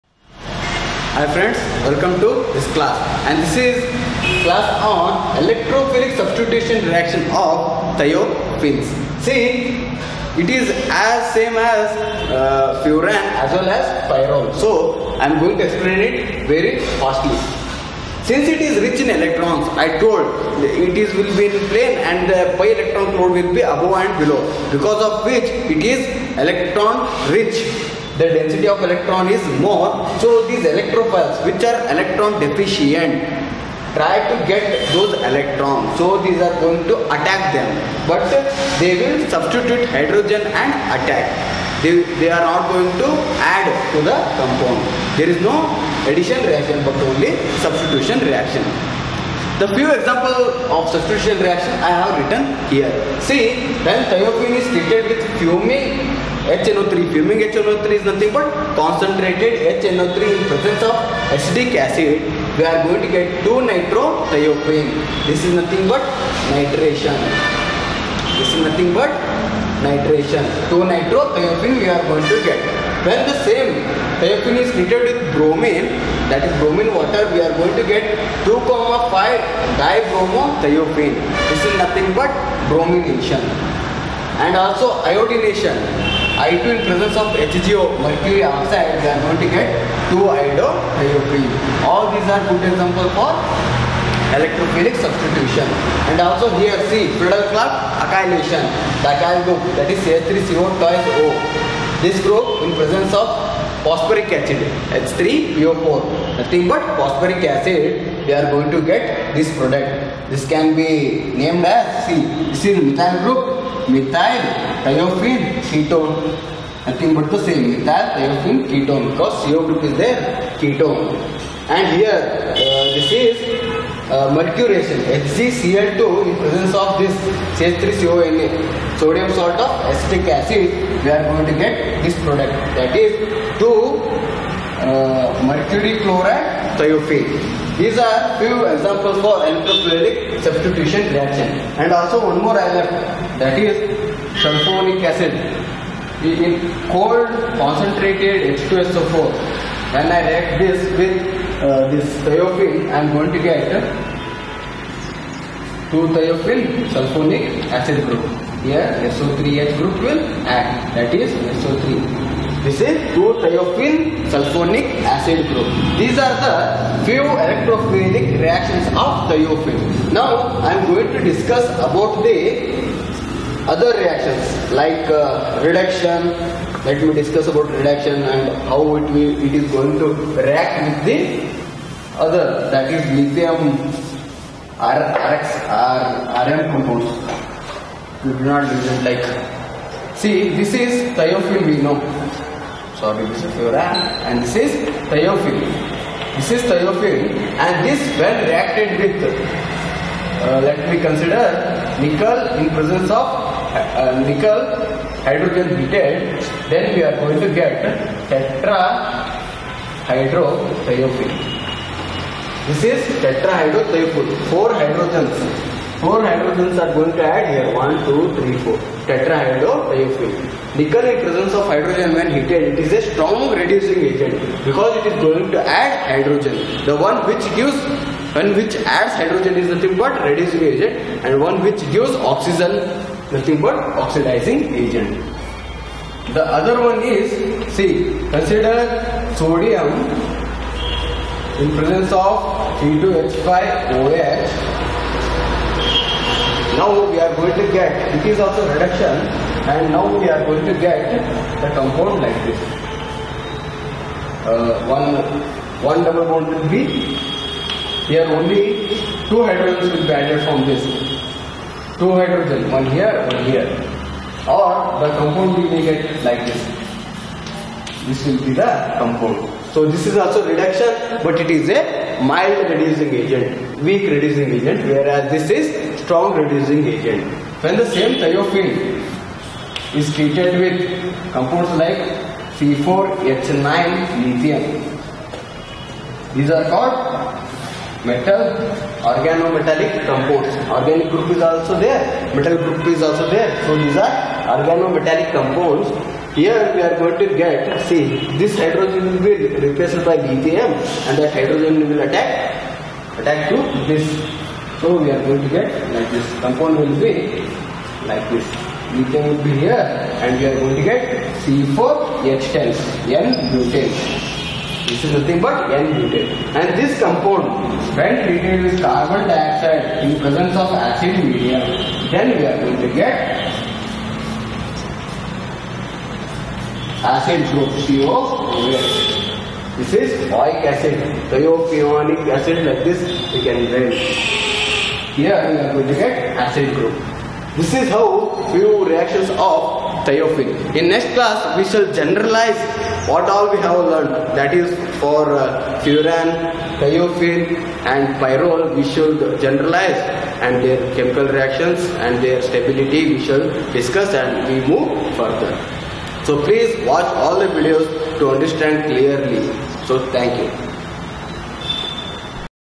12 Chemical Reactions of Thiophene
Electrophilic Substitution Reactions like Nitration, Sulphonation, Halogenation, Friedel-Craft Reactions
Reduction
Reaction with Organo-metallic Compounds